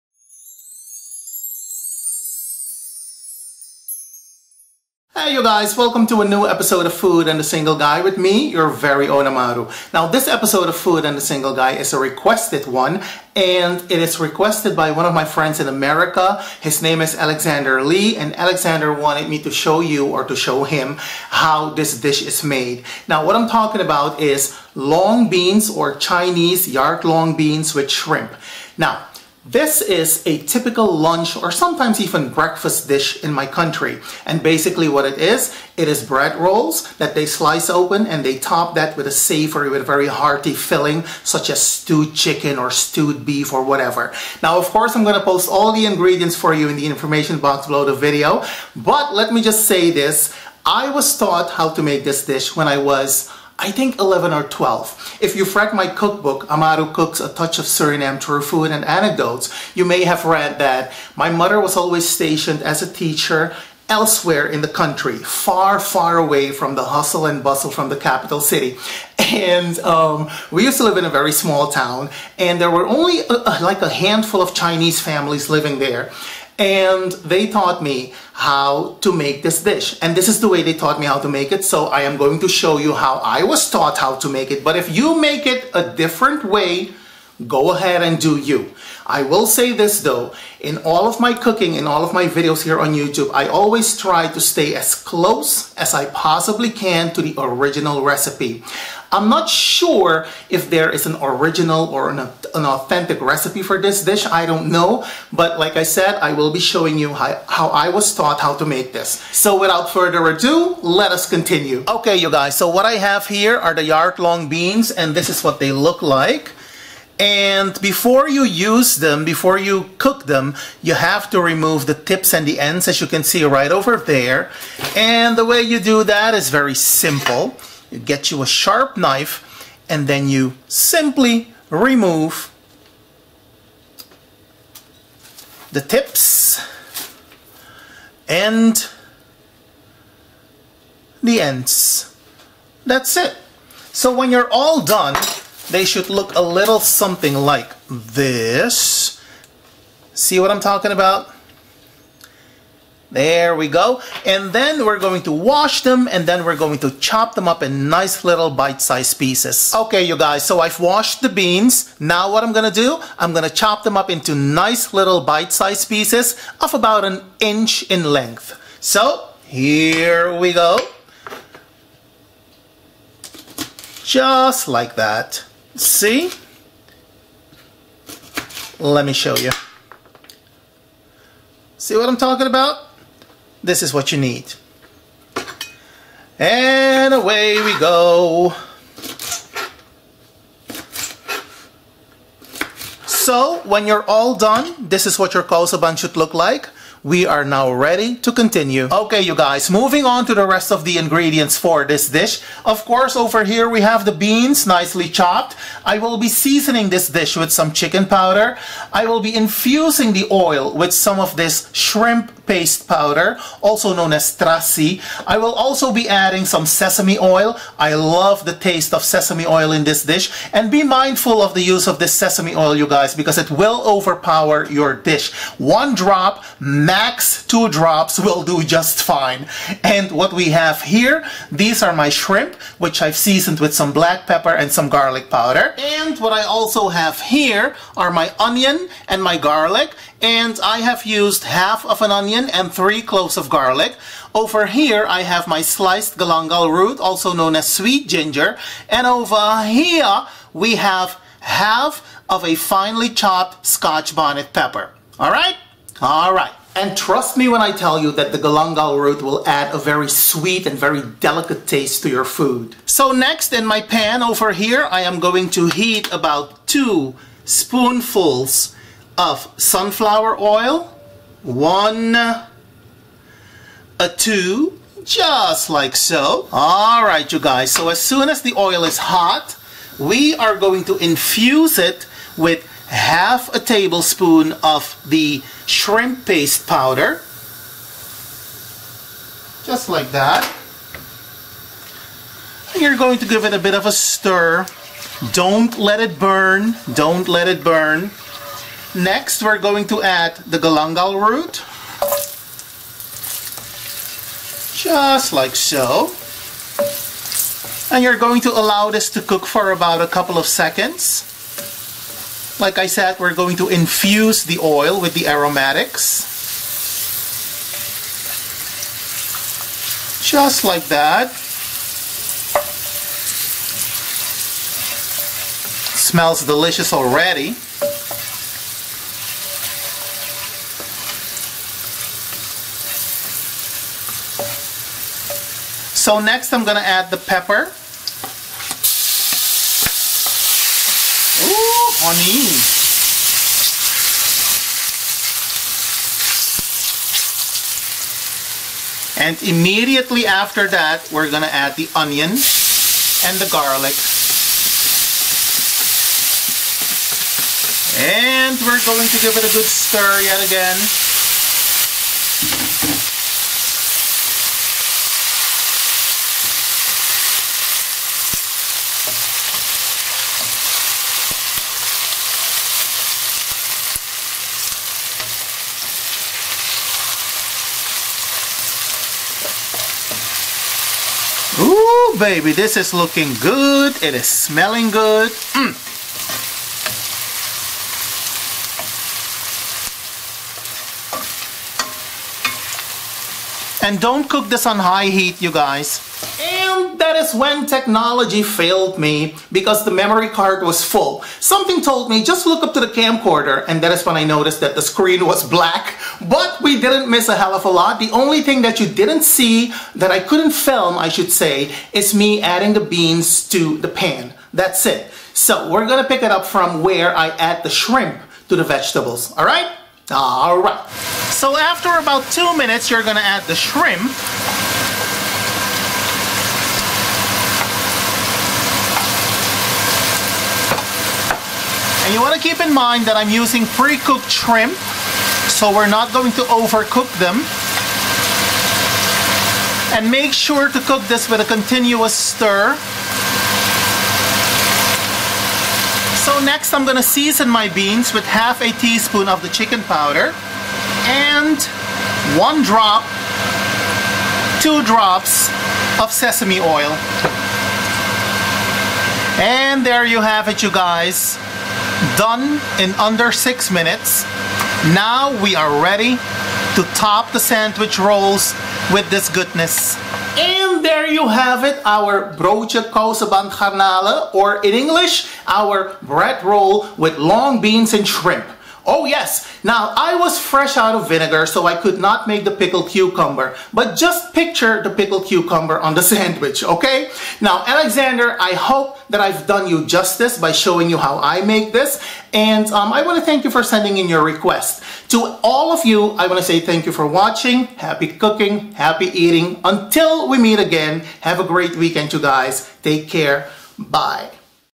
🇸🇷 Food & The Single Guy - S.11.10 - Broodje Kouseband Garnalen
On this episode: a requested dish by one of my viewers. A staple lunch dish from my homeland, these bread rolls topped with a hearty topping.

Ingredients:
- 3 small bushels of yard long beans (a.k.a. kouseband)
- 200 grams of small shrimp
- half of an onion
- 3-4 cloves of garlic
- half of a Scotch bonnet pepper
- chicken powder
- shrimp paste (or shrimp paste powder)
- galangal root
- garlic powder
- crushed black pepper
- vegetable oil
- few drops of sesame oil
- droplet of water (optional)

© AMARU MUSIC / AMARU PRODUCTIONS 2021